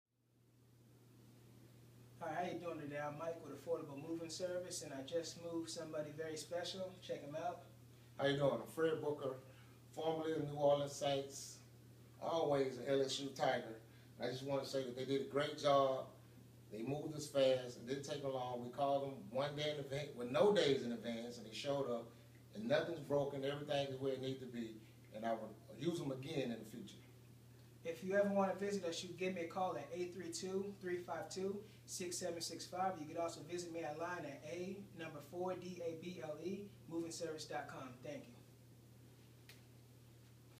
A4dable moving service with former NFL player Fred Booker
Watch as A4dable Moving Service gets good feedback from former NFL football player Fred Booker.